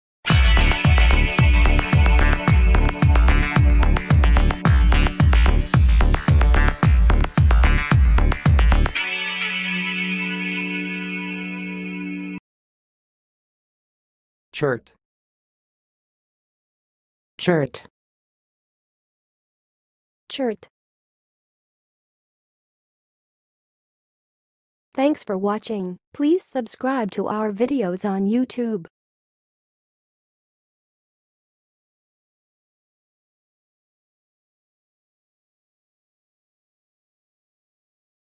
CHERTE - HOW TO PRONOUNCE CHERTE?
CHERTE - Speak like a pro: How to say Cherte in 2023

How do you correctly pronounce 'cherte'?
What are some alternative pronunciations for the word 'cherte'?
Can you provide audio pronunciations for 'cherte'?
What is the correct stress placement in the word 'cherte'?
Are there any specific phonetic features in pronouncing 'cherte'?
CHERTE Meaning And Definition:
A 'cherte' is a term used in Russian folklore to describe a supernatural creature or a witch-like figure.
It is associated with dark magic and is often depicted as an old hag with wrinkled skin and long, tangled hair.
CHERTE Pronunciation:
To pronounce 'cherte', say 'cher-teh'.
Other possible variations include 'chur-teh' or 'chehr-teh'.
Use CHERTE In Your Speech:
1. The cherte is often depicted as an old hag who lives deep in the forest
2. Legend has it that if you see a cherte, you must offer it a gift to escape its wrath
3. In Russian folklore, the cherte is known for its cunning and mischievous nature
4. The cherte is said to have the ability to shape-shift into various animals
Please answer in comments for the most popular CHERTE questions:
1. What are some other supernatural creatures in Russian folklore?
2. How is the cherte different from other witches in folklore?
3. What kind of magic is associated with the cherte?
4. Are there any specific rituals or offerings associated with the cherte?
5. What are some famous stories or legends involving the cherte?
6. Do people still believe in the existence of the cherte today?